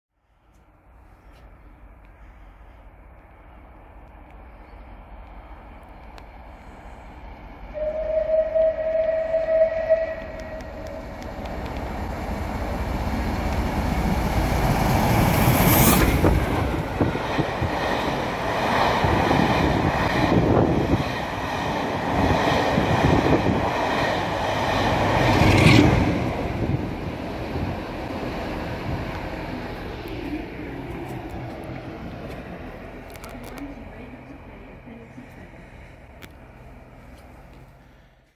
34046 Braunton whistling through Grateley - 28/11/23
Yes I got a face full of smoke,  but it smelled wonderful :-)